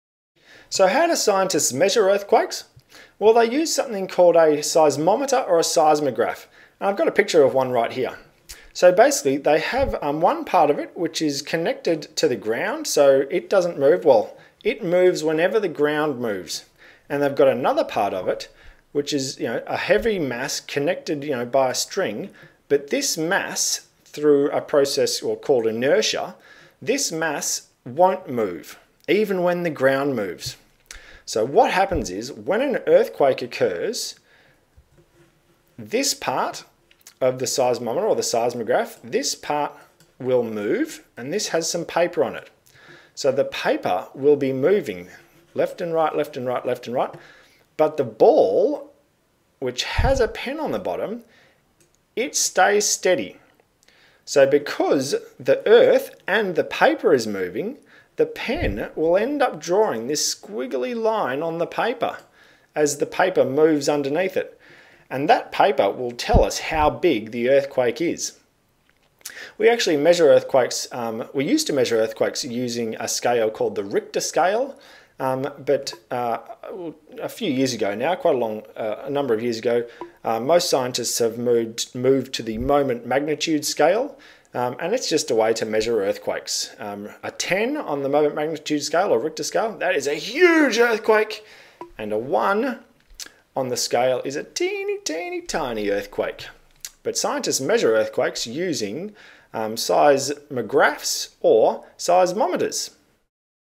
Measuring Earthquakes
An explanation of the way a seismograph works.